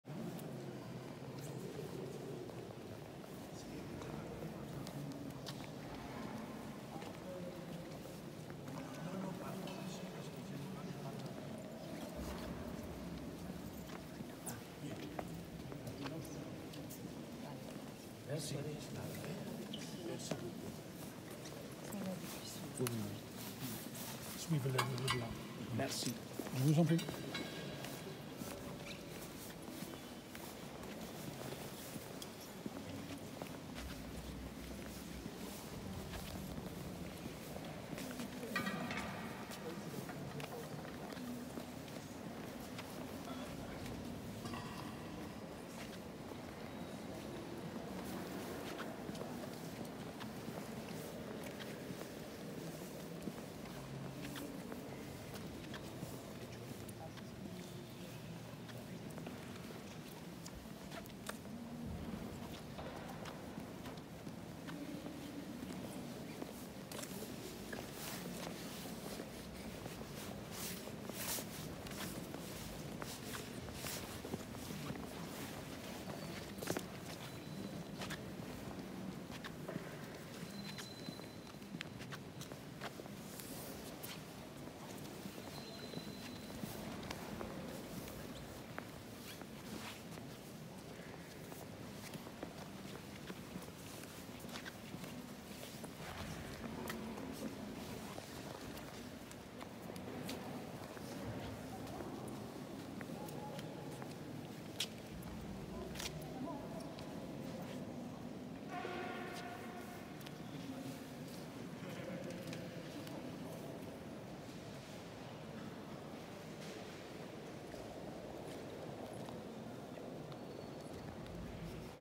Chartres Labyrinth Wandering – step by step. Walking the labyrinth in Chartres Cathedral.
The last labyrinth wandering for the 2024 season. A bit of a Mr. Bean moment.  When a body meet a body...

Walking the labyrinth in Chartres Cathedral.
Join us on a serene and meditative journey through the labyrinth at Chartres Cathedral. This iconic Gothic cathedral, located in Chartres, France, is home to one of the most famous labyrinths in the world. Walk with us as we explore the intricate pathways and delve into the spiritual and historical significance of this ancient practice. Perfect for those interested in pilgrimage, meditation, and the beauty of historic architecture.